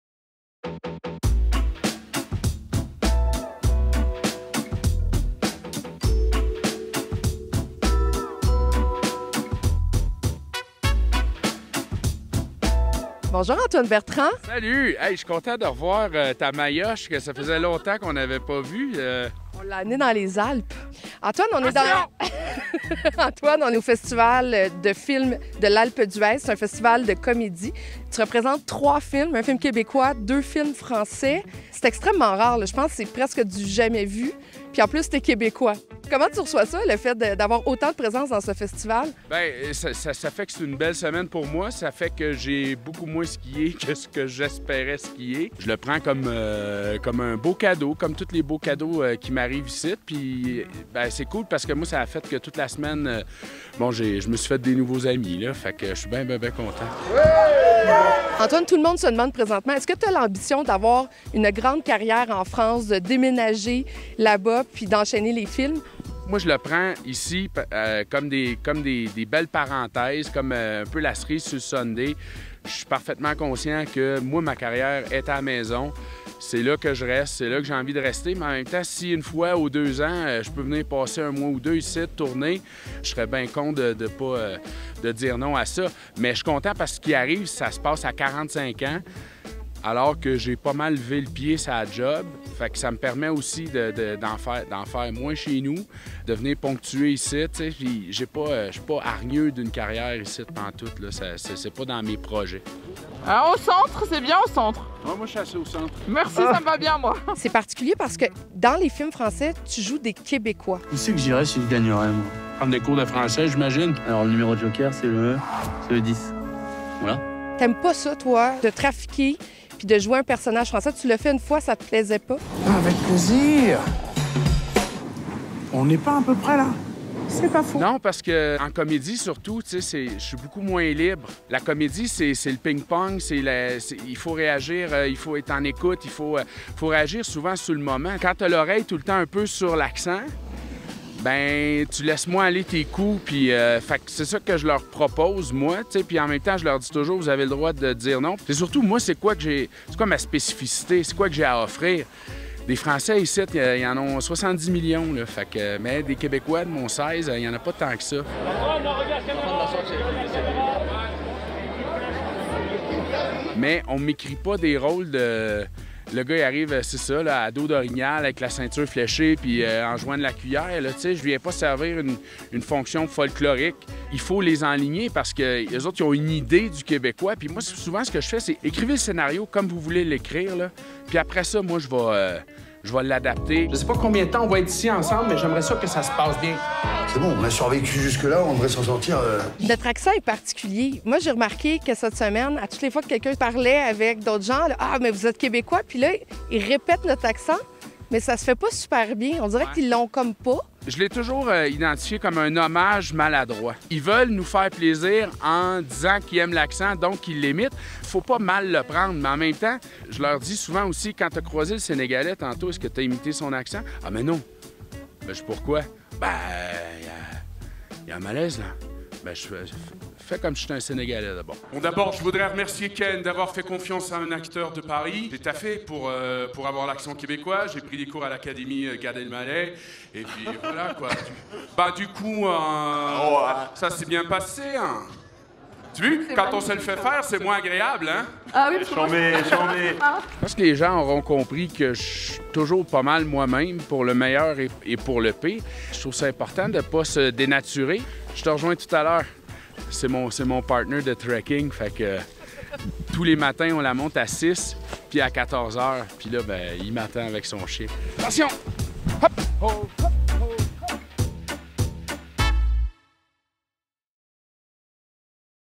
Antoine Bertrand et ses projets français
Antoine Bertrand a participé à plusieurs projets de films français. Lors de son passage récent au Festival de l'Alpes d'Huez, il représentait deux films français et un film québécois. Il a donc conquis le coeur des festivaliers français avec son authenticité légendaire et ce, sans trafiquer l'accent québécois. Rencontre au sommet avec l'acteur québécois.